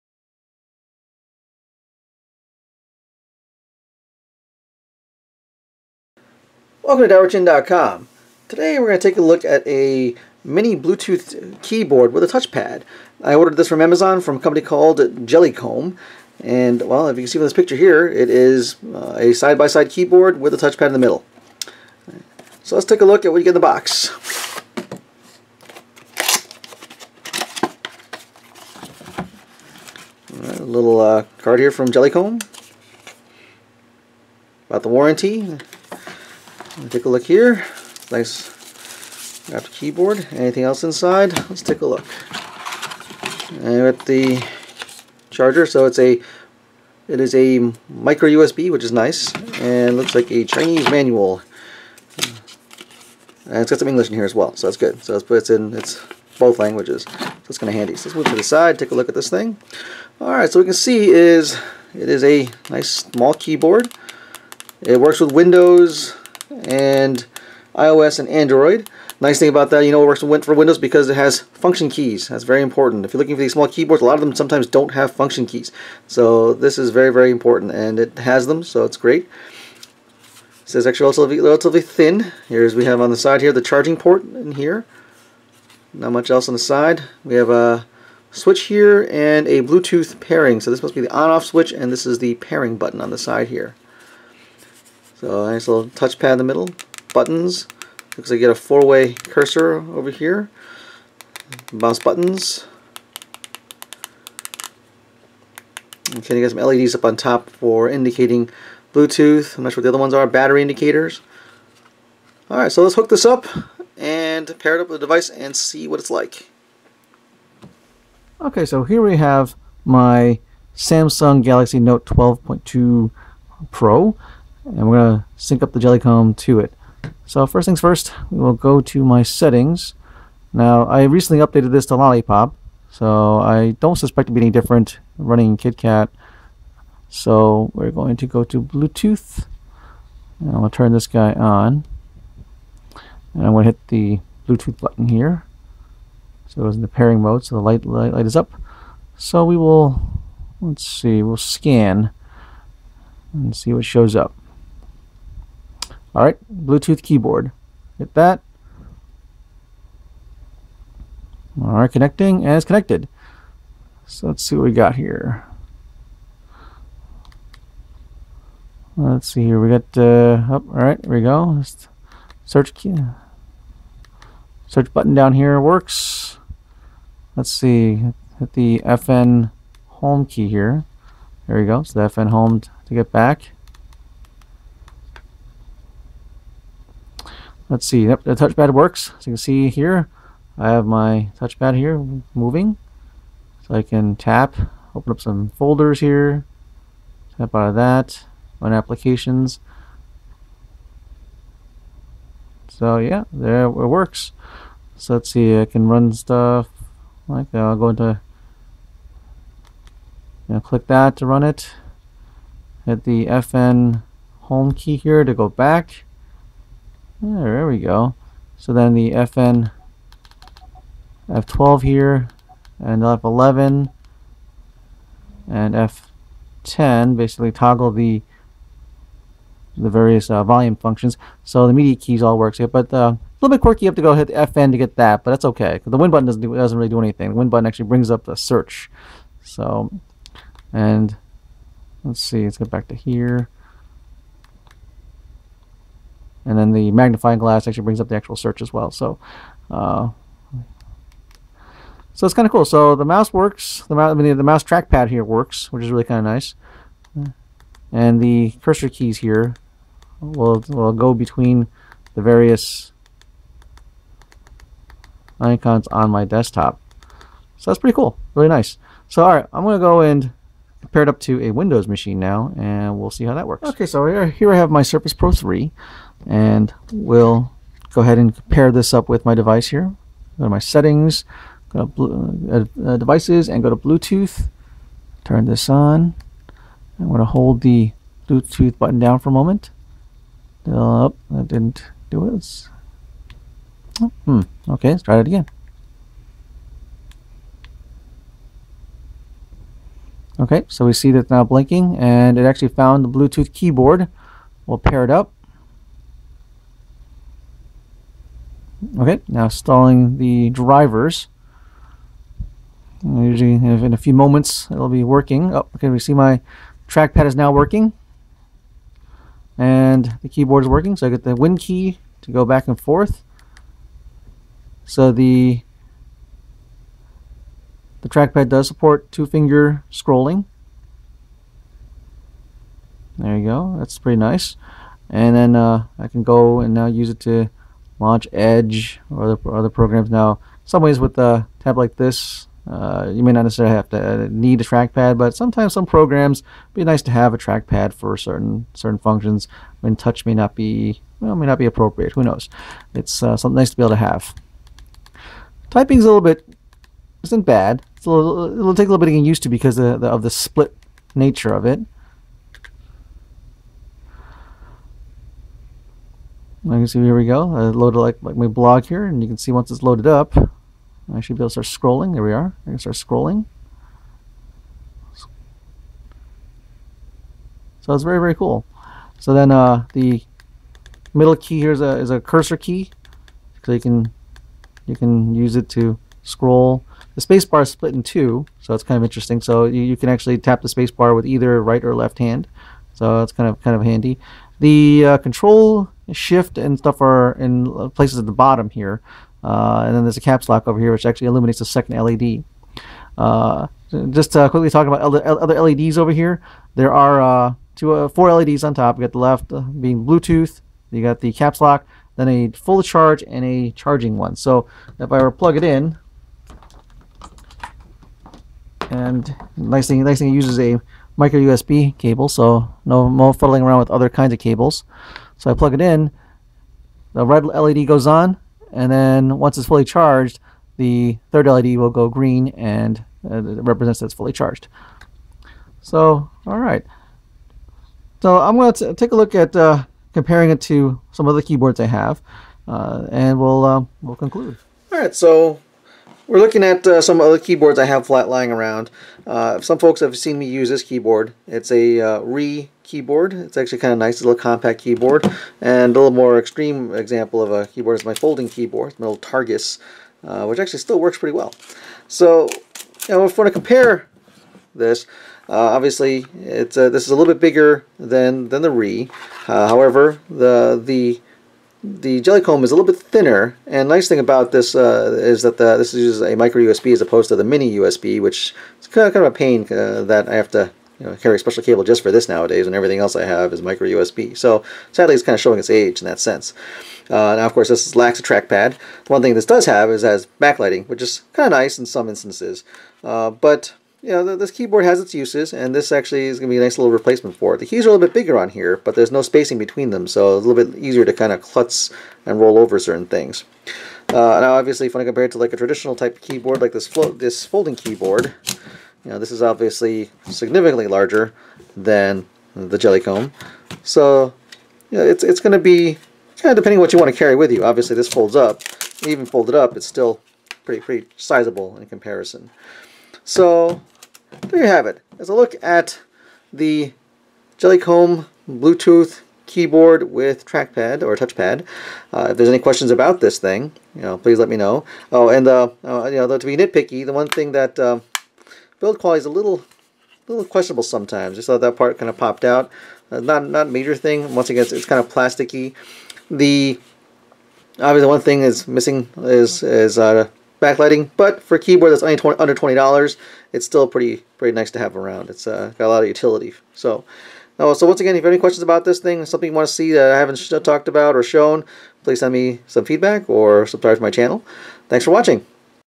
Jelly Comb Mini Bluetooth Touchpad Keyboard Review
As much as Windows 10 and other devices move toward touch interfaces, there are plenty of applications out there that still need a keyboard. Typing lengthy articles or emails demands the use of a keyboard (at least for me). Older applications, especially on the Windows side will still work better when paired with a keyboard, and with the advent of small Windows tablets, it's important to have one with you. Bulk is a concern, so I always look to find small companion keyboards to carry along. Sometimes, it's nice to have a mouse/pointer device to go along too, but that usually means sporting an external mouse. Luckily I stumbled across this device, sold by Jelly Comb on Amazon. It's a nifty bluetooth keyboard with an integraded touchpad. It's inexpensive, but seems to do the job. Take a look at the video and see for yourself if this Mini Bluetooth keyboard is something that should go along with your tablet.